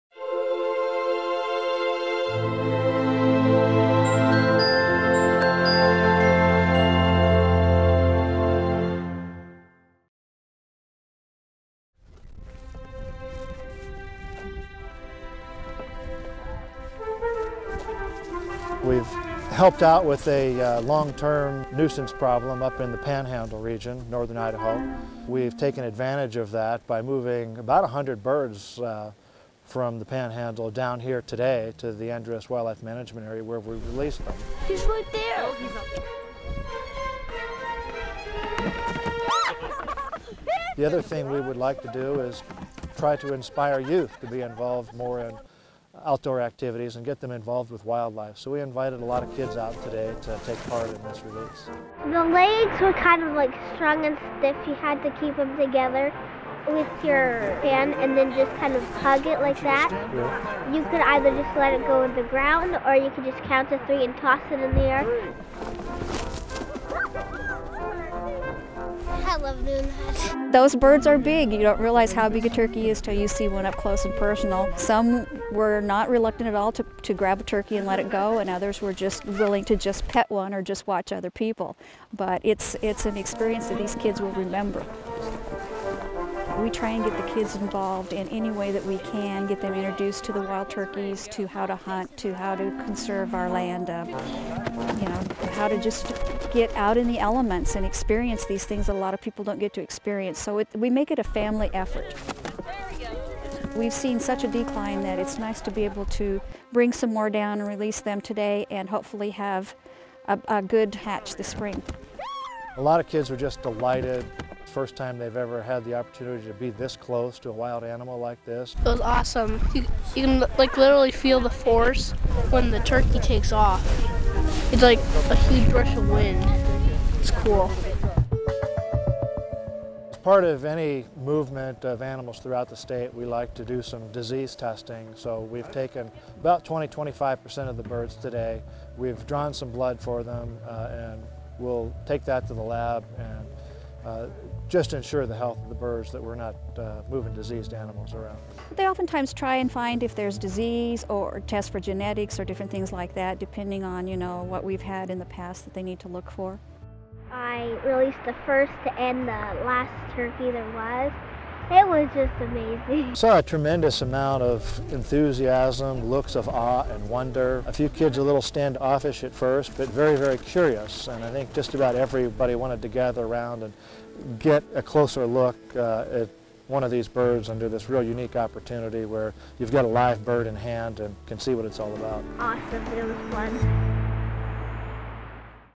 Volunteering: Turkey Release
Volunteers help to release turkeys into new areas.